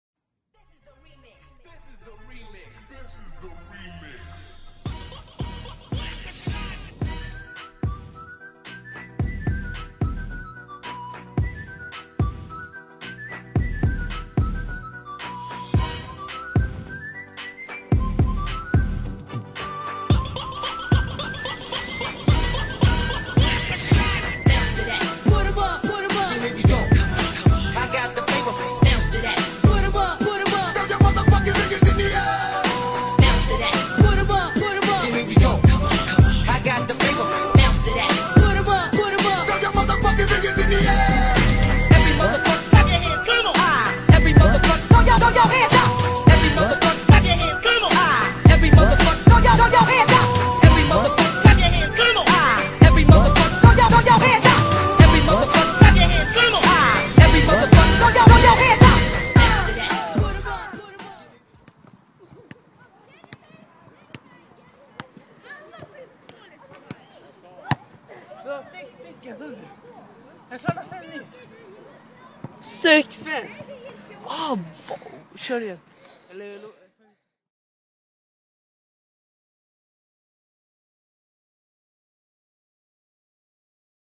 Strömvallen •Day - 1• With - Jakob Kindbom
Me and my friend having fun on Strömvallen,
My firs video of  "Strömvallen Day-"

4/4 - 2013